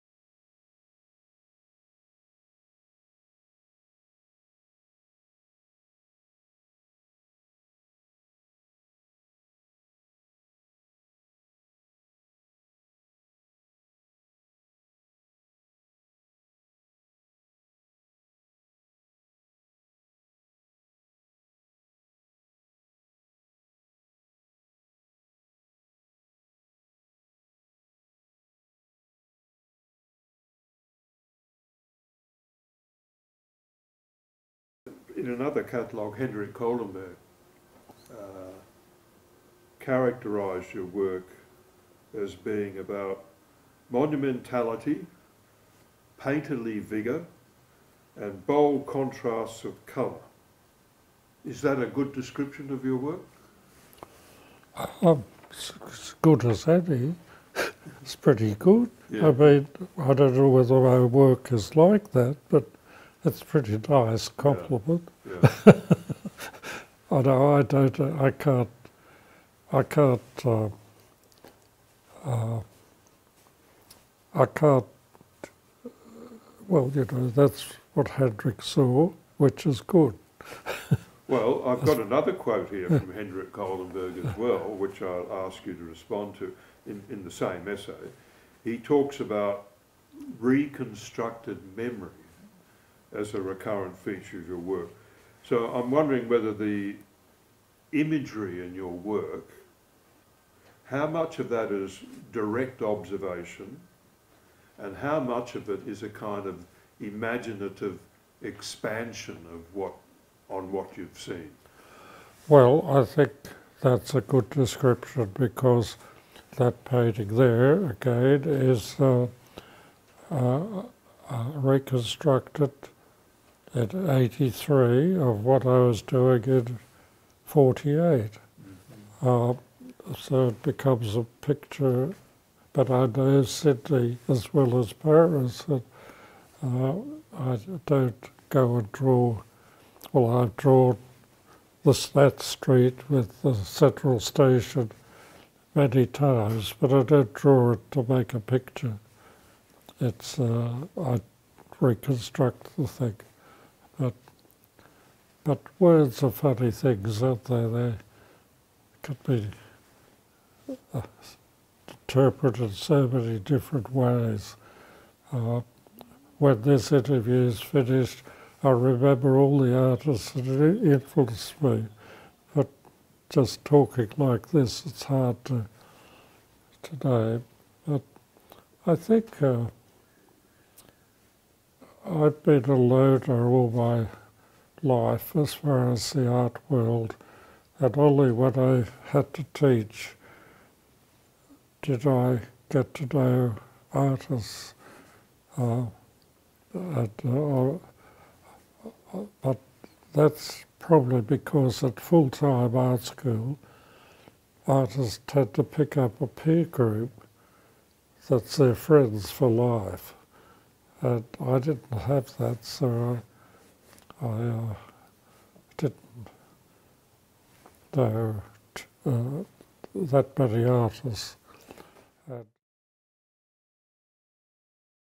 Kevin Connor in conversation with Paul McGillick
This is an excerpt of a recorded conversation between artist Kevin Connor and Paul McGillick. This is part of the Cultural Conversations project.